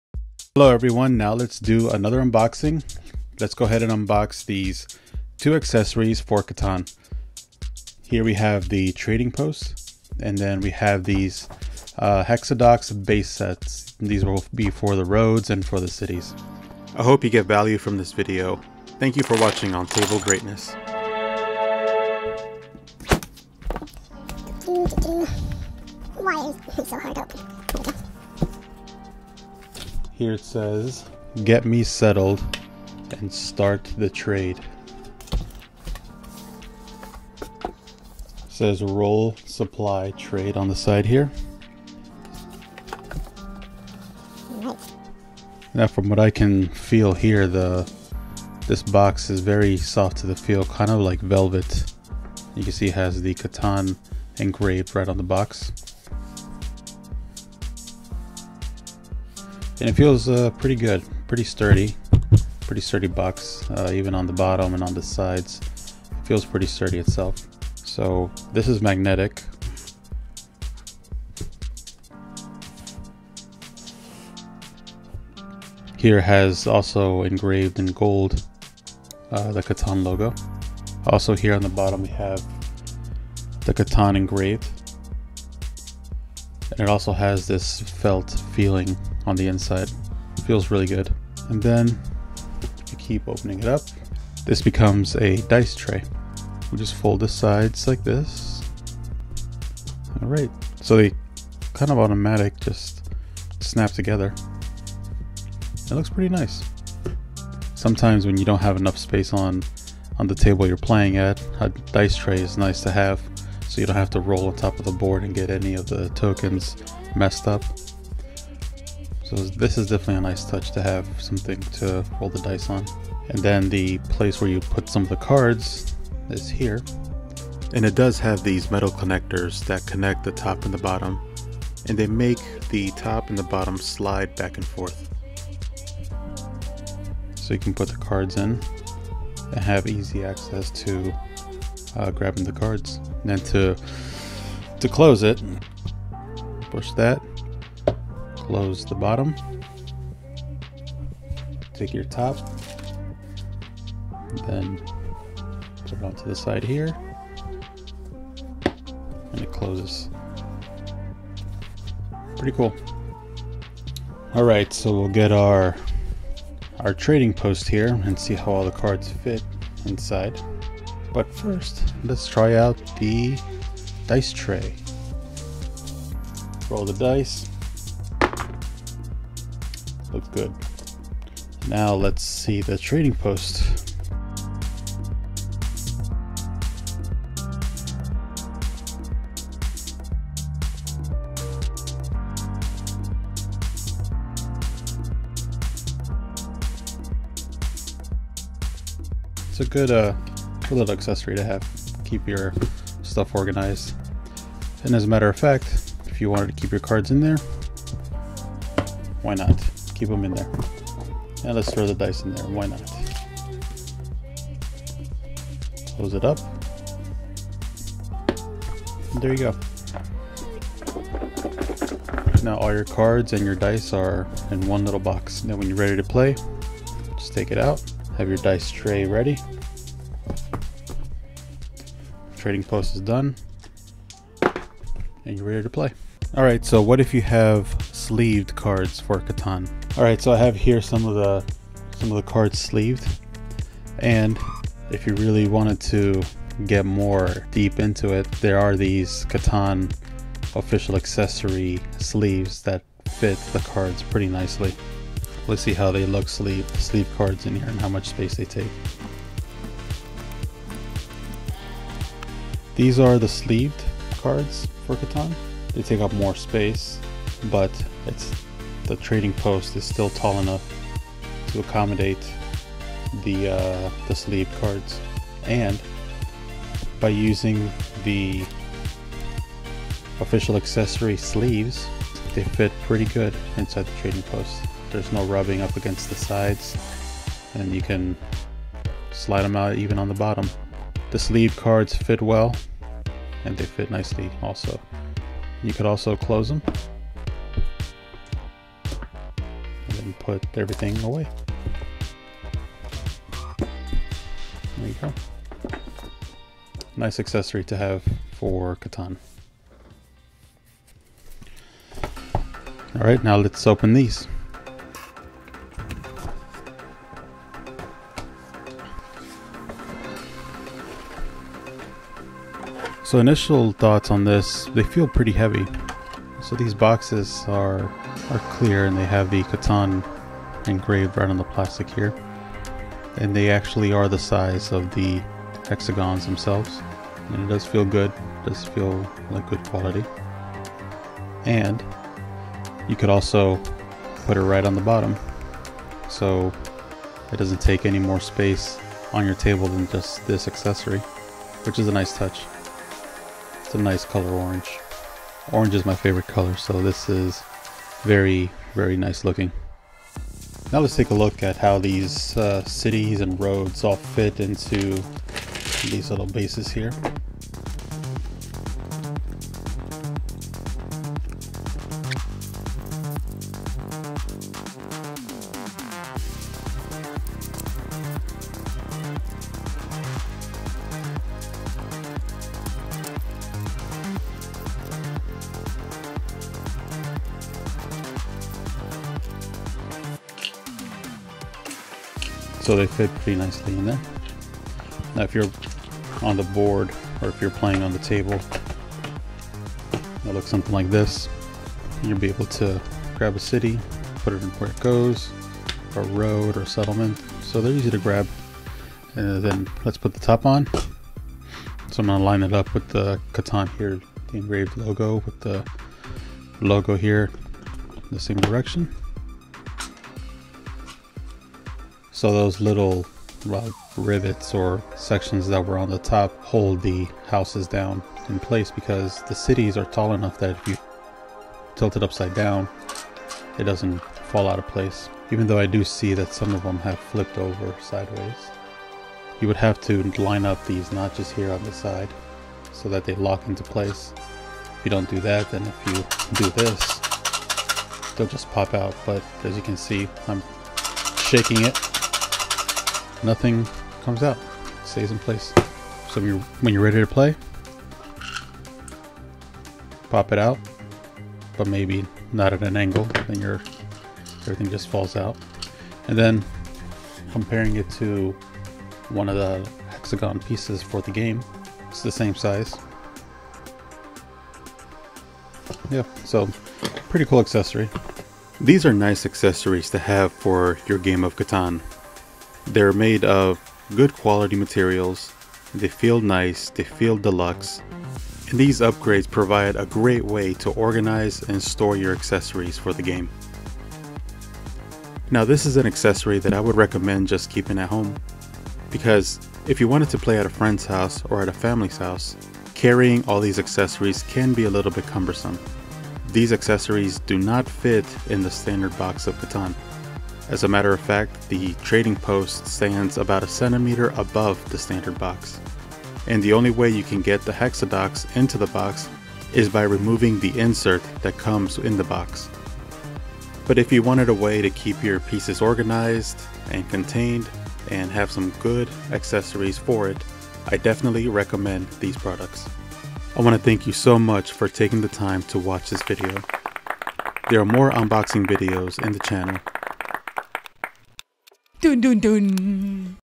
Catan Trading Post and Hexadocks Game Accessories - Unboxing and Review! SPOILER: REAL NICE!
In this video we unbox and review two accessories for the Catan boardgame.  The trading post is used to store the resource cards and the hexadocks bases hold the streets, settlements and the cities.

0:00 Introduction
0:23 Trading Post Unboxing
2:43 Trading Post Functionality
5:11 Hexadocks Unboxing
6:00 Hexadocks Functionality
7:58 Final Thoughts